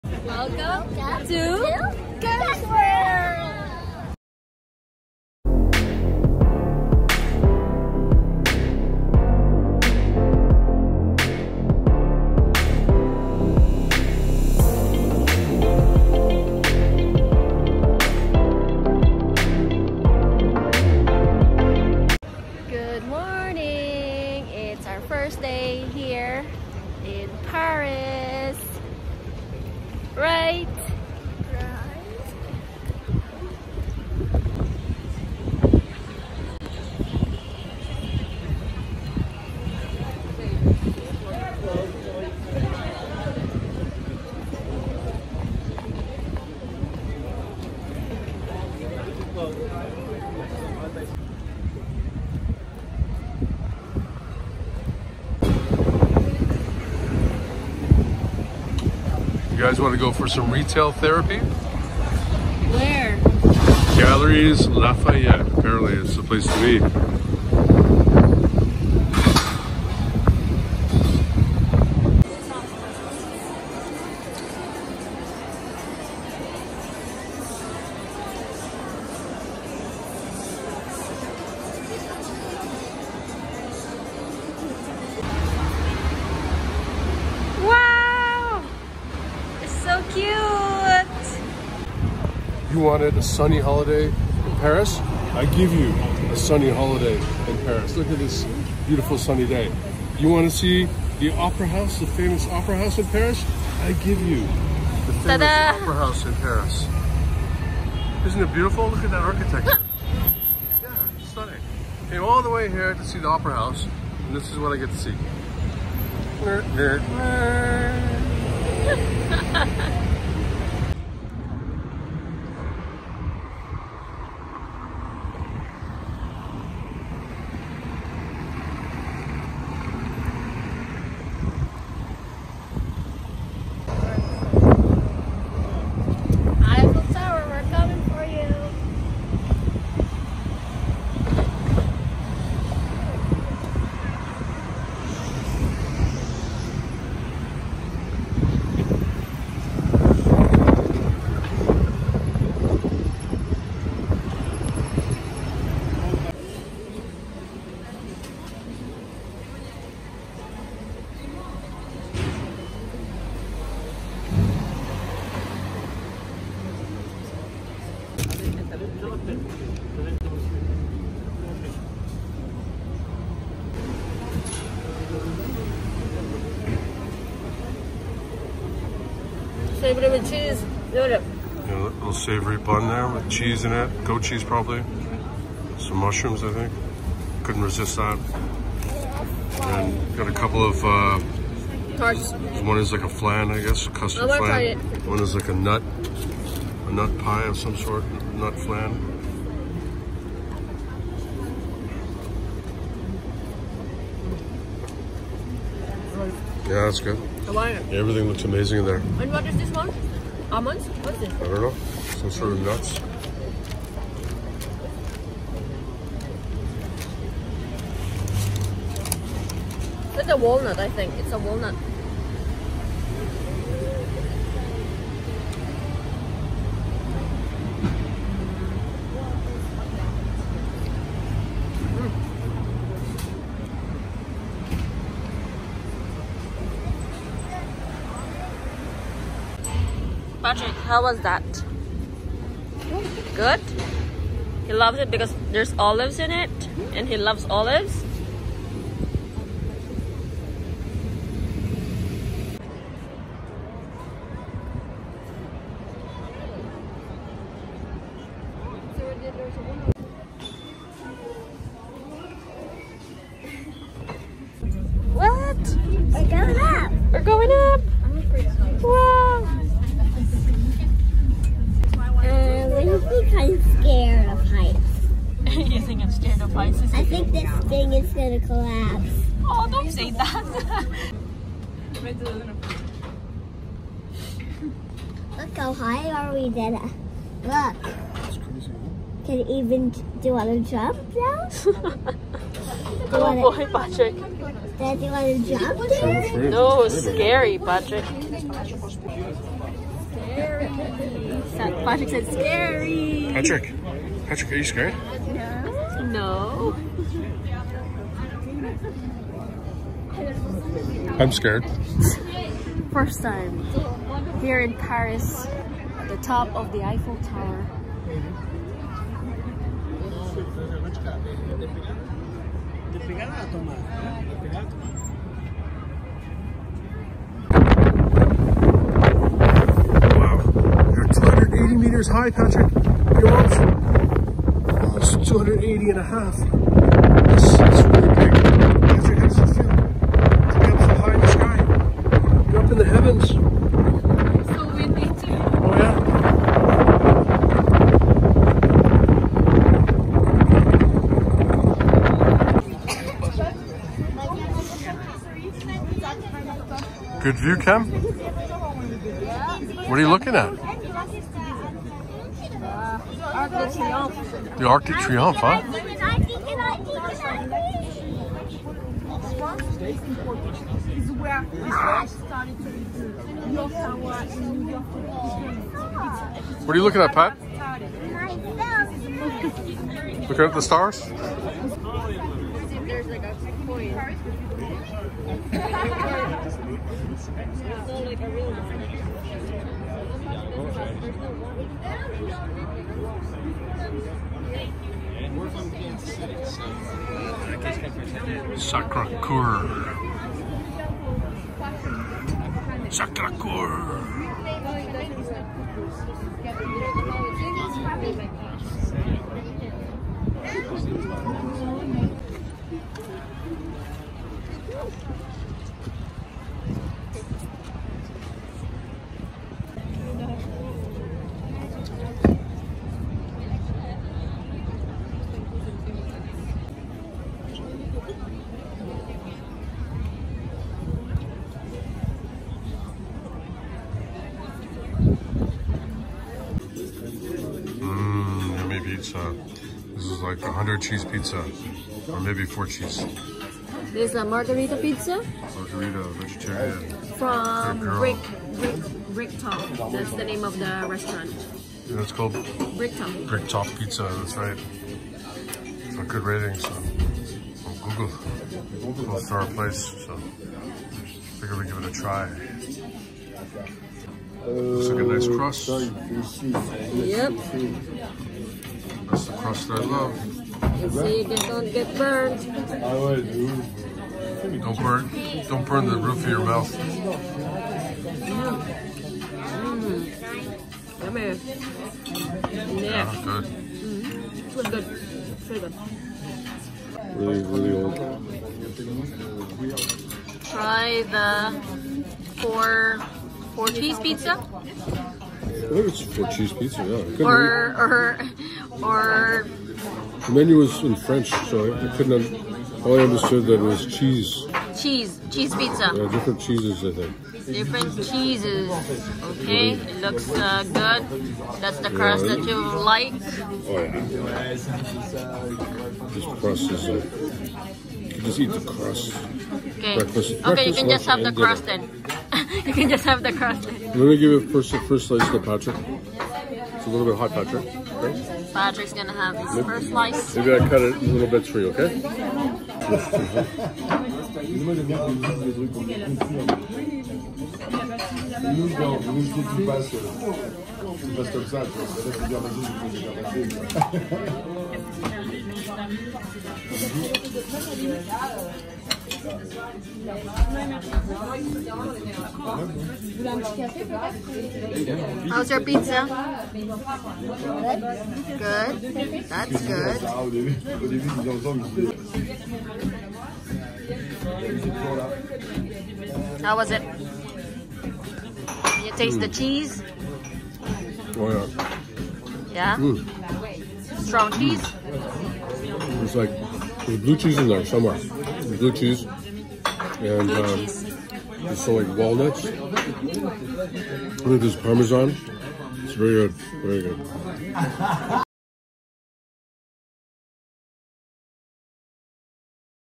Day 1 | Eiffel Tower, Maison Bergeron, Bricktop
It’s our first full day in Paris. We decided to go for a good long walk to Eiffel Tower.

While on the way to the Eiffel tower, we passed the Ministry of Justice square, check the Galeries Lafayette, and stop over at Maison Bergeron for coffee and some pastries.

We are able to go at the top of the Eiffel tower and see the amazing view.

Finally ending our day by having pizza at Brickstop. It was delicious 💜